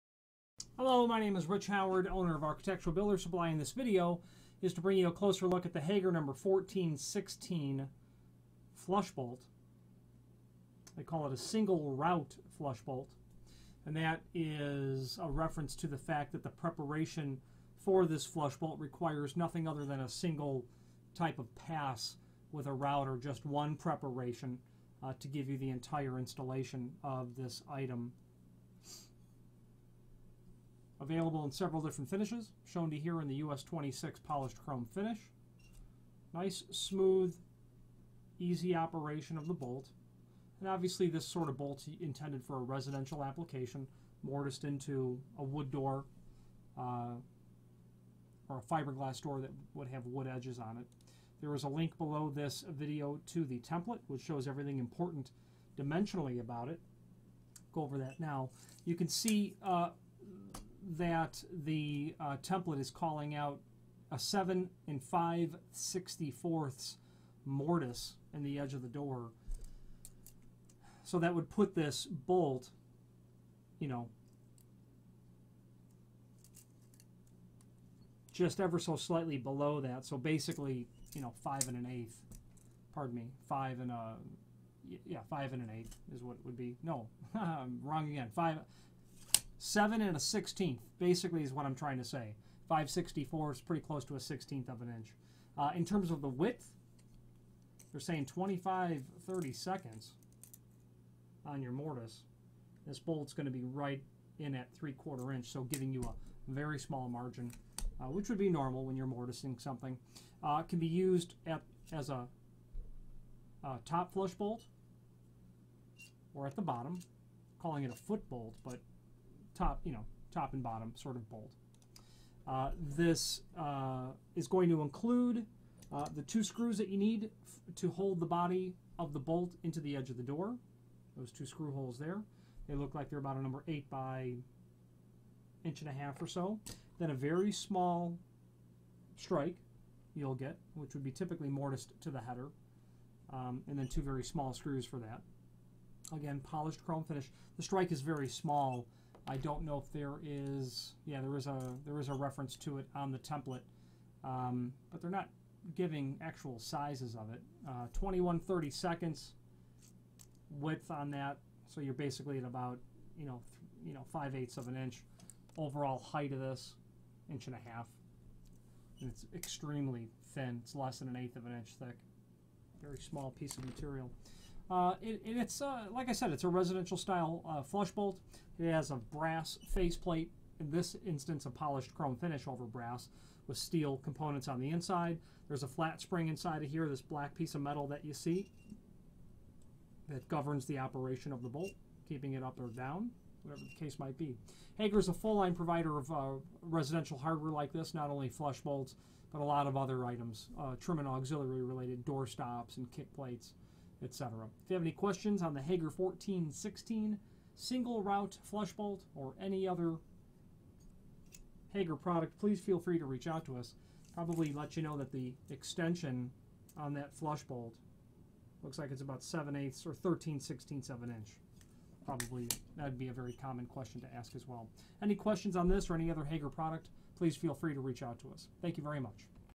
Hager 1416 - US26 - Bright Chrome Single Route Flush Bolt
This video is to bring you a closer look at the Hager 1416 - US26 - Bright Chrome Single Route Flush Bolt.
Single route is in reference to the preparation of this item (single pass with a router).
Available in several different finishes.
This item used primarily used in residential applications (wood doors).
Link below this video to the template that shows everything important dimensionally about the item.
The template is calling out a 7-5/64" mortise in the edge of the door.
The width is 25/32".
The extension on the bolt is 13/16".
This can be used as a top flush bolt or at the bottom.
Includes two screws for holding the body of the bolt onto the edge of the door.
Small strike is included and two small screws (typically mortised to the header).
Dimension of the strike: 25/32" width, 1-1/2" overall height, less than an 1/8" thick.
Polished chrome finish over brass material; steel components on the inside.
The flat spring controls the operation of the bolt.
Hager is a full-line provider of items like this and much more.